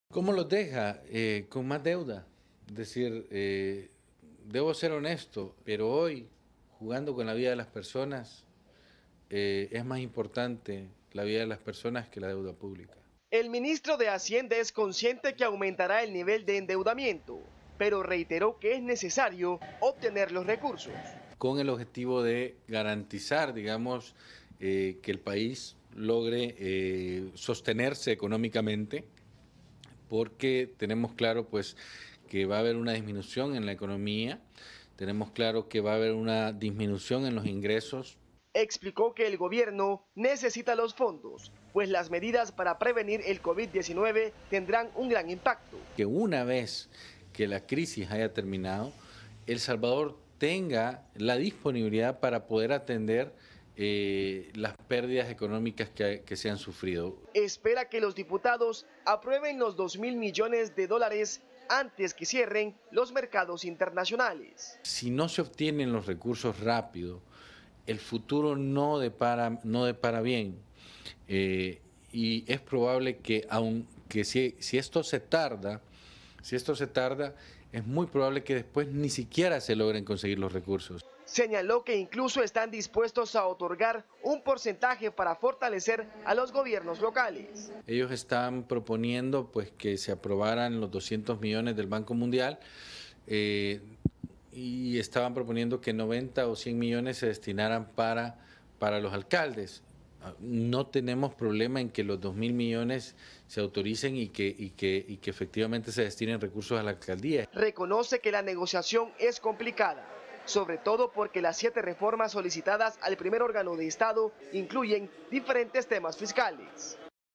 Teleprensa33 | Ministro asegura que urgen los fondos
El ministro de Hacienda, Nelson Fuentes, urgió a los diputados para que aprueben los 2 mil millones de dólares que ha solicitado para hacer frente a la crisis generada por la emergencia del covid-19.

Nelson Fuentes, ministro de Hacienda

Viernes 20 de marzo del 2020